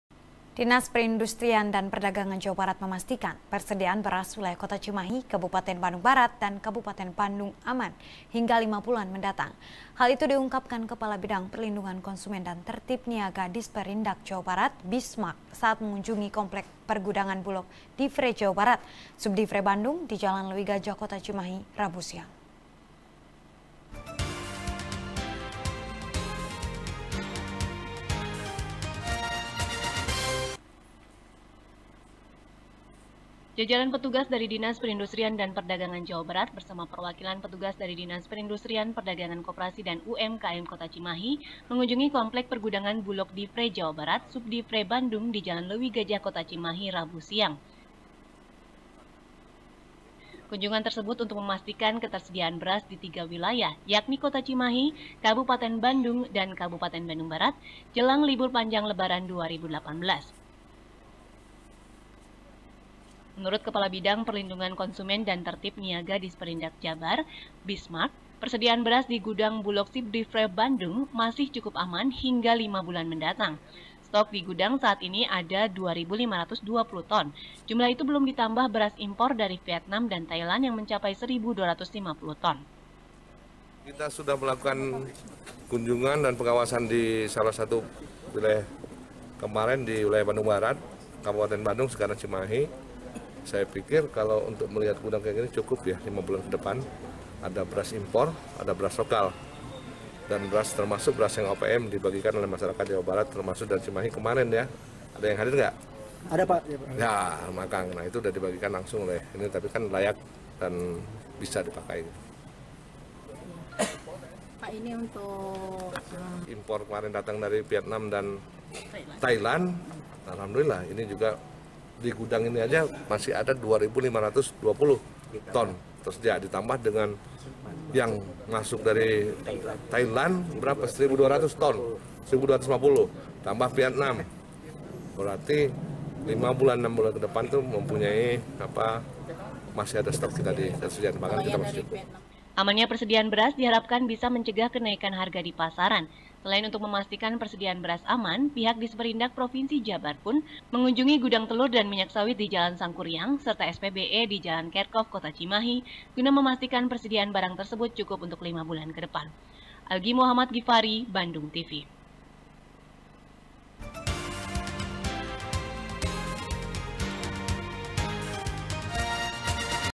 Disperindag Jabar Pastikan Stok Beras Aman | SBR | BANDUNG TV
SBR | BANDUNG TV | NIKEN PRATIWI |Dinas Perindustrian dan Perdagangan Jawa Barat memastikan, persedian beras wilayah kota Cimahi, kabupaten Bandung Barat, dan kabupaten Bandung aman, hingga lima bulan mendatang. Hal itu diungkapkan kepala bidang perlindungan konsumen dan tertib niaga Disperindag Jawa Barat, bismark, saat mengunjungi komplek pergudangan Bulog divre Jawa Barat, Subdivre Bandung, di jalan leuwigajah kota Cimahi, Rabu siang.